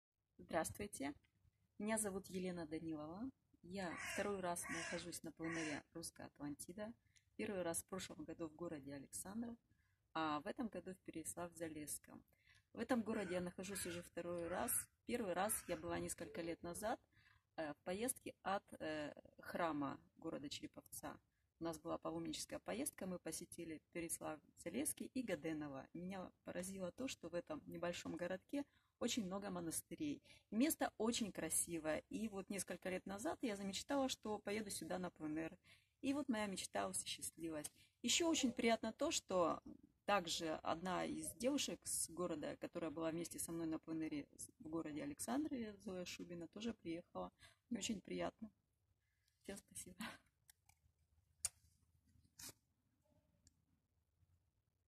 Работа на пленэре в Переславле-Залесском
Художница Елена Данилова делится своими впечатлениями